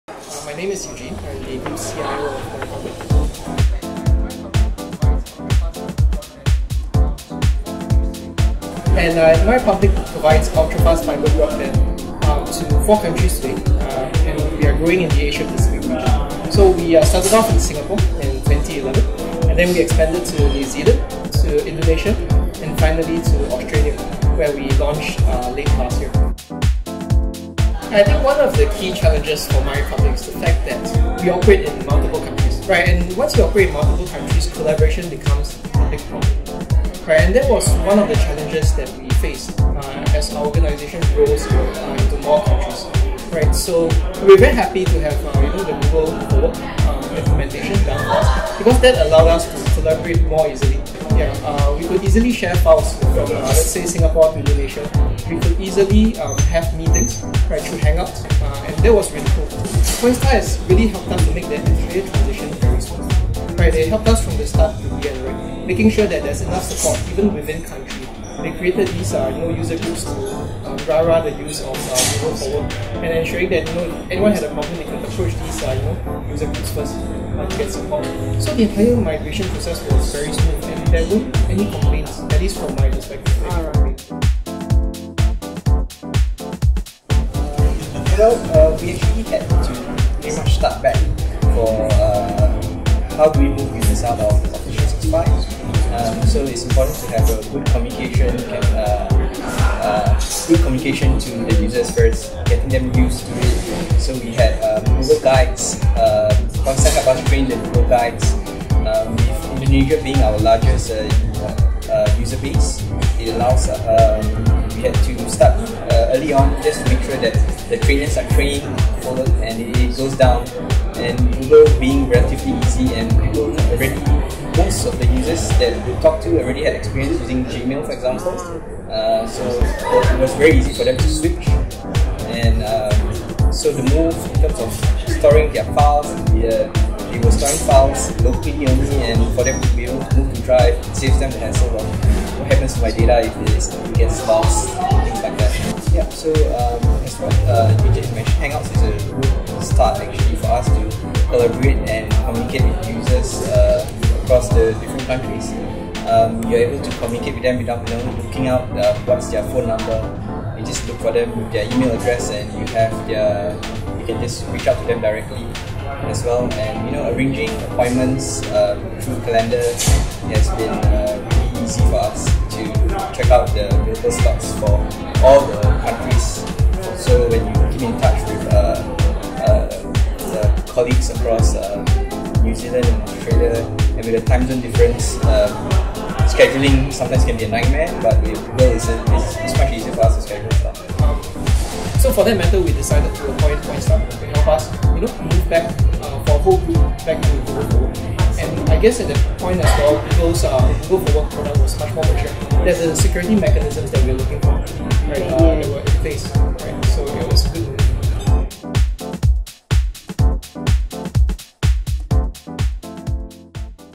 Consolidation of MyRepublic’s Multiple Subsidiaries Made Easy with G Suite
Since 2011, MyRepublic has been empowering thousands of homes and businesses with ultra-fast fiber optic internet broadband in not only Singapore but Indonesia, Australia and New Zealand.

With their multi-location day-to-day operation, it suffices to say that It can be a struggle to collaborate so they decided to migrate to G Suite for enhanced security and optimized real-time collaboration.